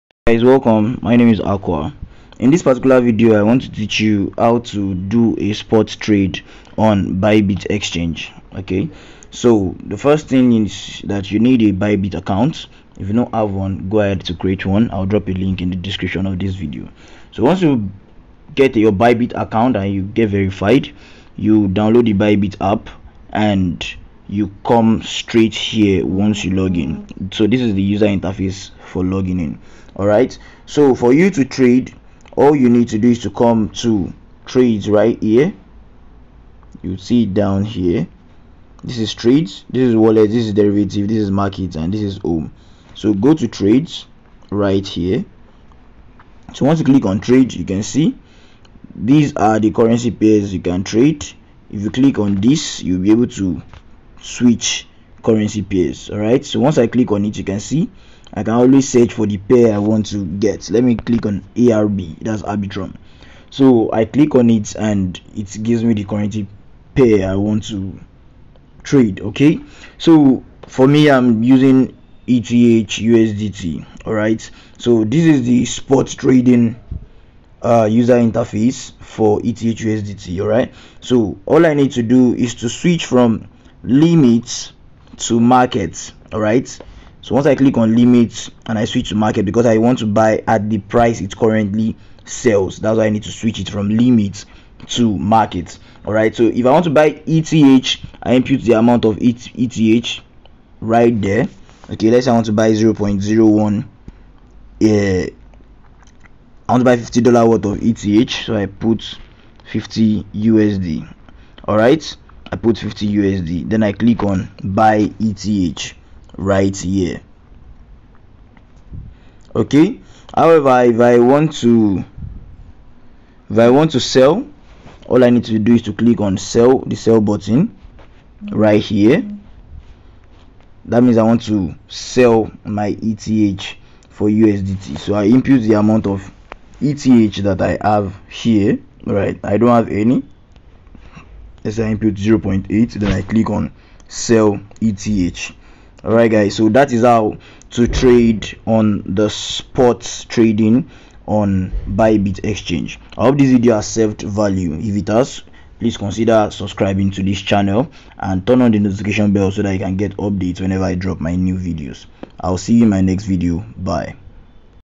How to Buy and Sell Crypto on ByBit Spot Trade
To trade spot on ByBit Exchange, you will need to follow these steps:

Sign up for an account on ByBit Exchange and complete the KYC process

Deposit funds into your account using one of the supported cryptocurrencies or fiat currencies.

Navigate to the "Spot Trading" section of the exchange.
Choose the cryptocurrency pair you want to trade.

Enter the amount you want to buy or sell and place your order.

Monitor your order to see if it gets filled and adjust it if necessary.